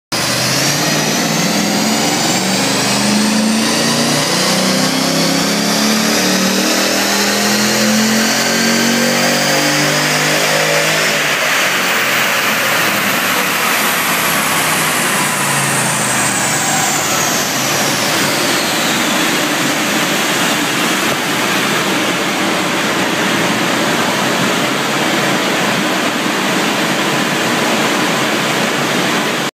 VE E3 Maloo - Exhaust, Ramjet & Tune = 299 rwkw
To this E3 HSV MY12 Maloo we fitted Di Filippo 1 3/4 Headers, 3" Hi Flow Cats, 3"Cat Back System, Ramjet Cold Air Intake and gave it a full custom tune for a result of 299 rwkw